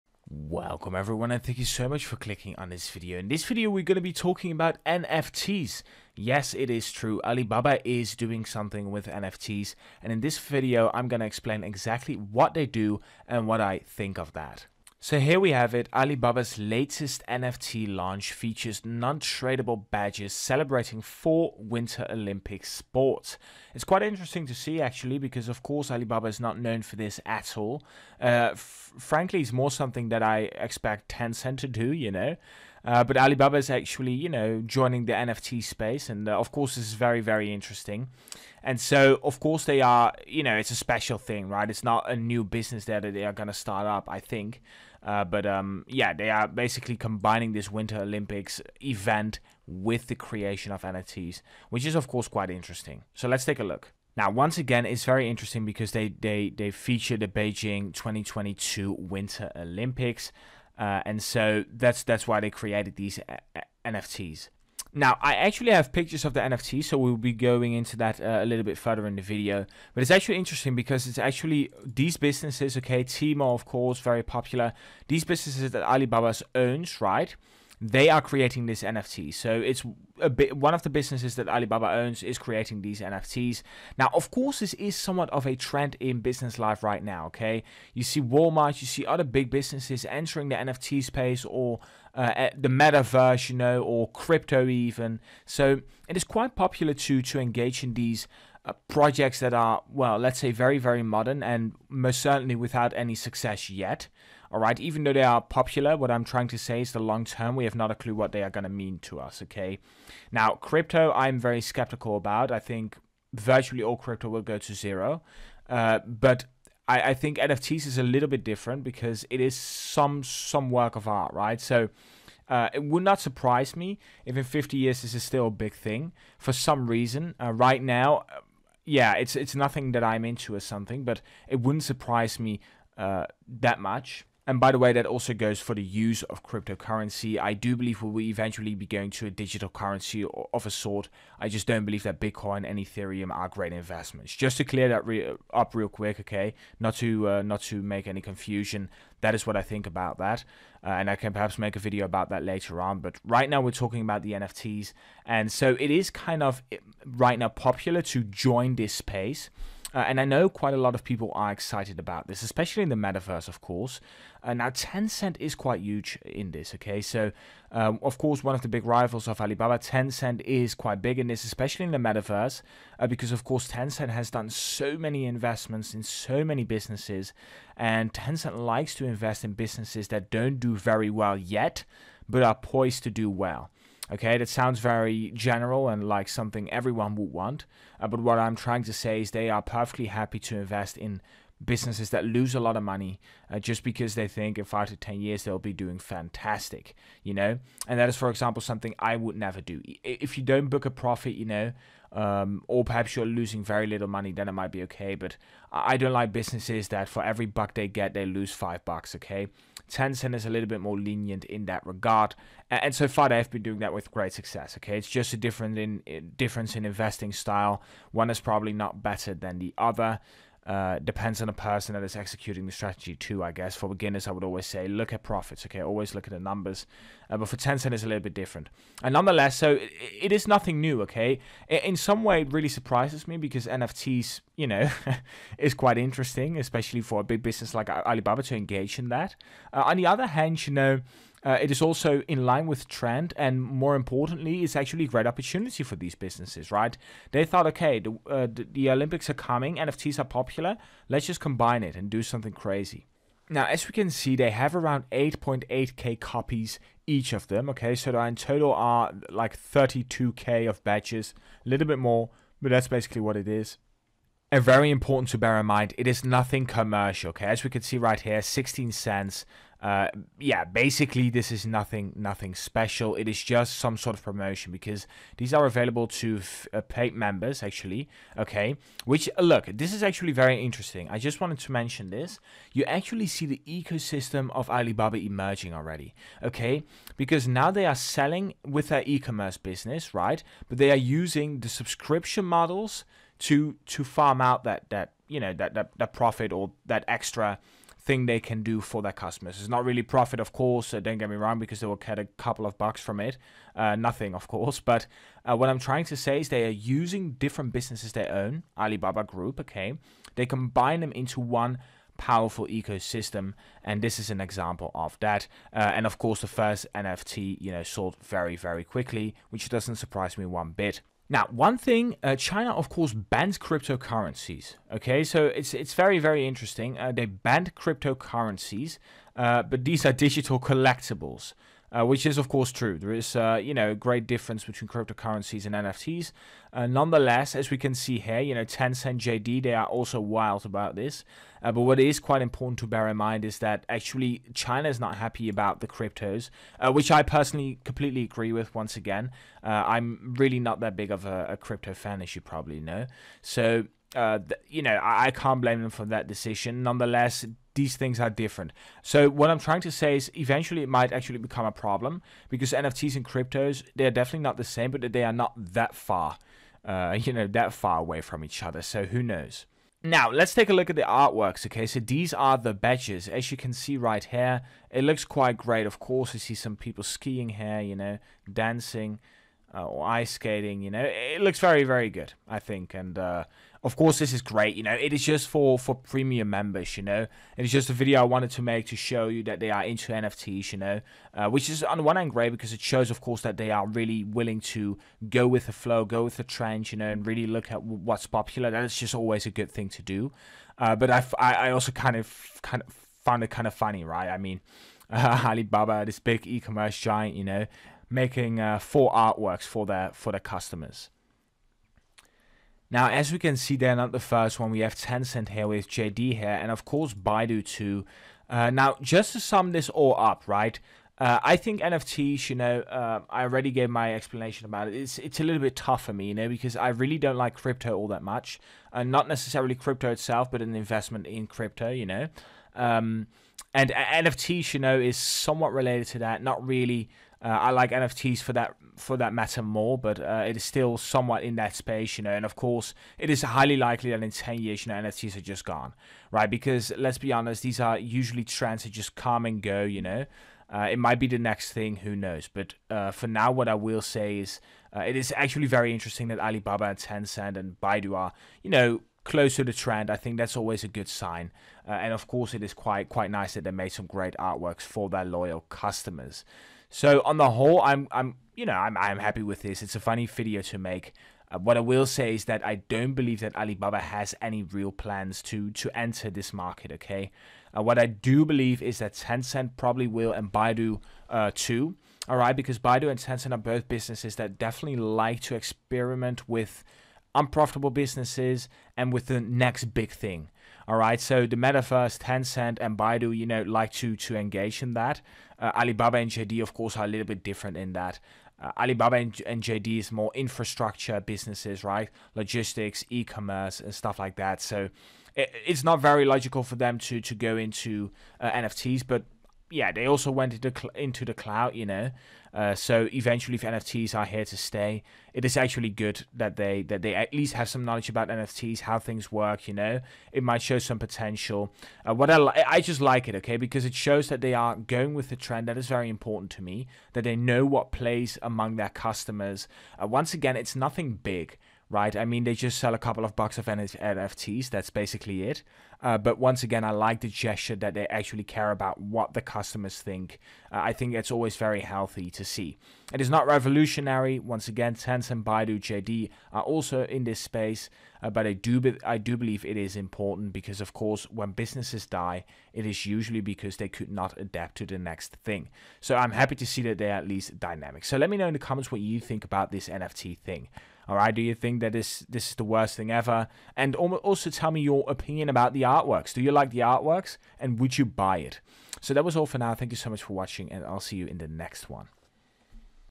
ALIBABA IS EXPLORING NFTS! | Alibaba Stock Analysis | Alibaba Stock News | Alibaba Stock Forecast
0:00 Introduction
0:14 Alibaba's Launch into NFTS
0:53 The Details

Today we'll take a look at Alibaba who is entering the NFT space. Yes that's right, after JD and Tencent, Alibaba is now also active in the NFT industry. Is this doomed to fail or is it a great opportunity?